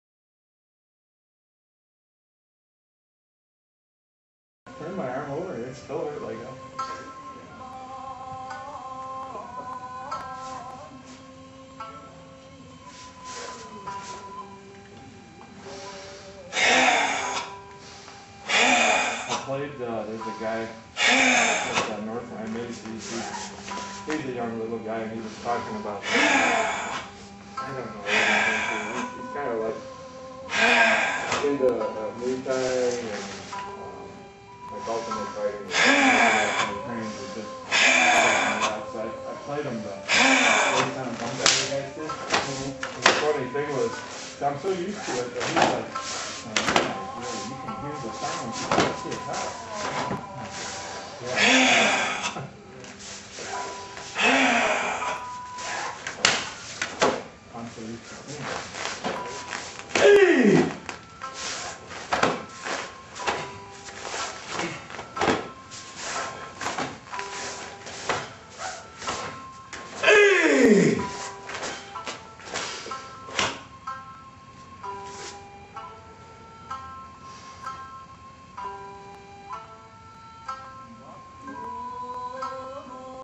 Isshinryu Seiunchin Kata
Isshinryu's Seiunchin Kata performed by Jeff Perkins, 7th Dan & Chief Instructor of Okinawa Budo Kai Livonia, Michigan.